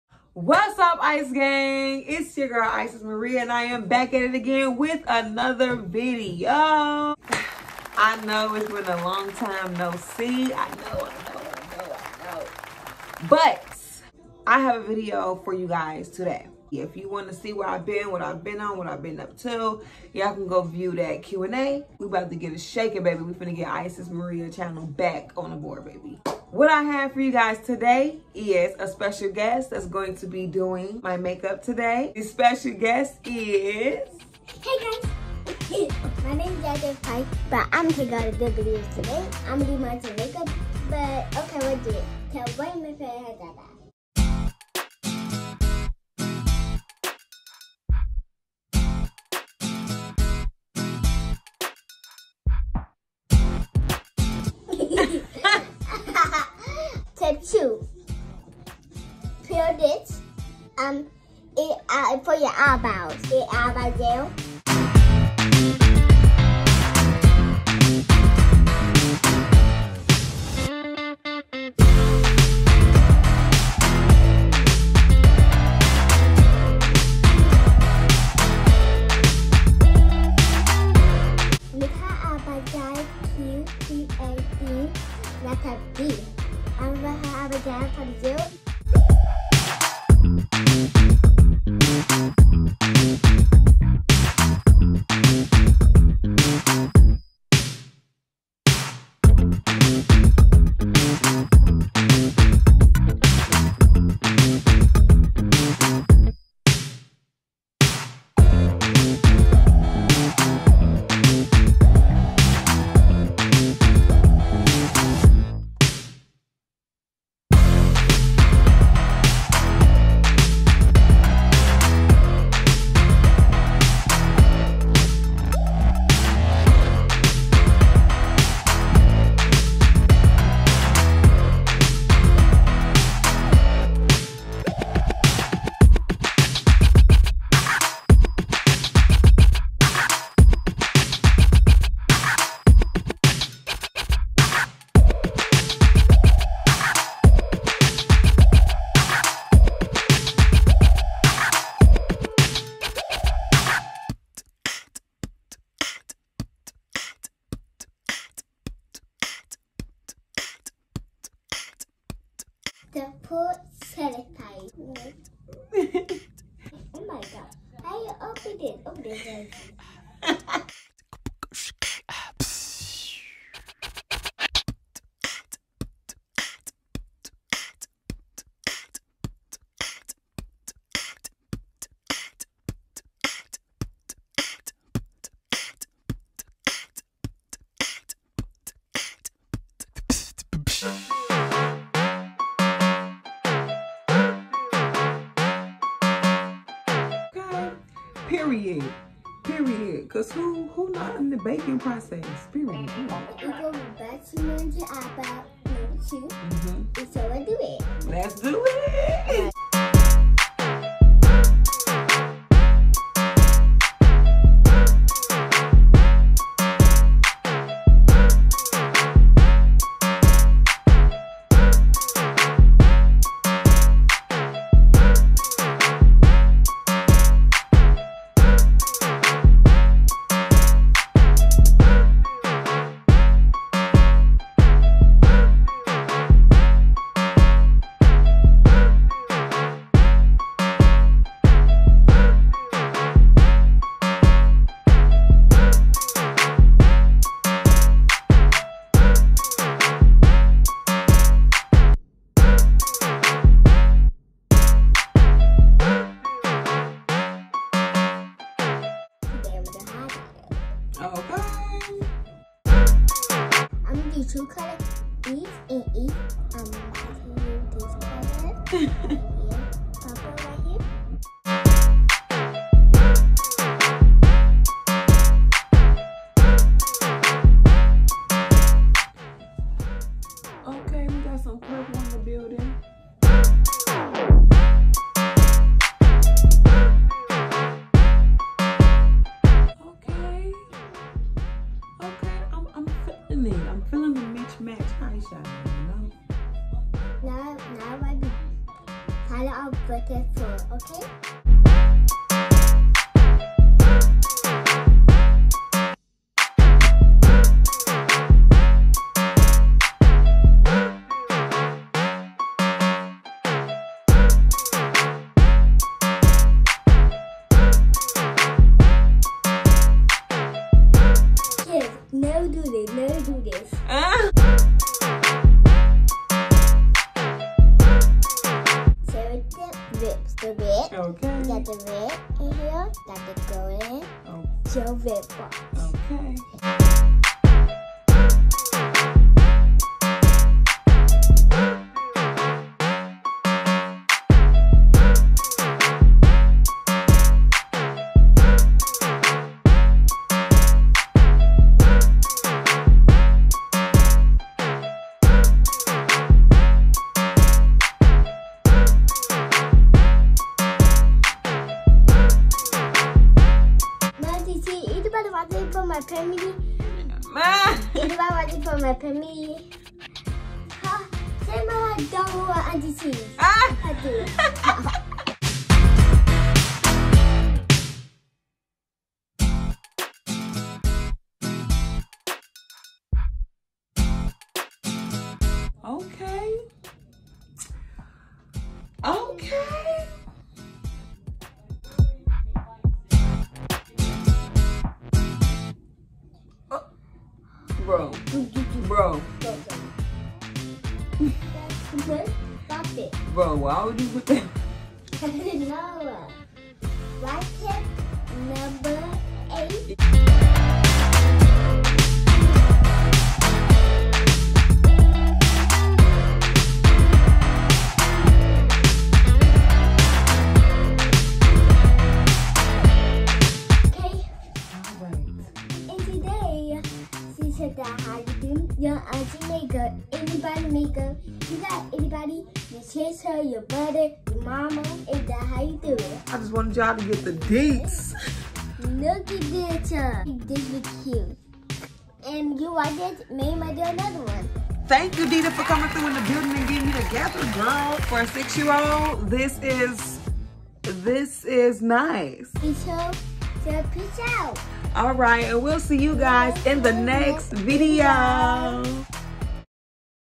⚠️ 1ST VIDEO BACK IN 11 MONTHS ⚠️ Deeda does my makeup 😱💄 (check description box)
Wassup ICEGANG! Moving forward, the description box is where I’ll leave materials I use in videos or information I’d like for you guys to have! Enjoy!!

Materials used in the video (found in local beauty supply)
1. Hand sanitizer
2. Eyebrow gel
3. Brown eyebrow powder
4. Foundation
5. Concealer
6. Setting powder
7. Eyeshadow
8. Mascara
9. Lipstick
10. Lipgloss
11. Setting spray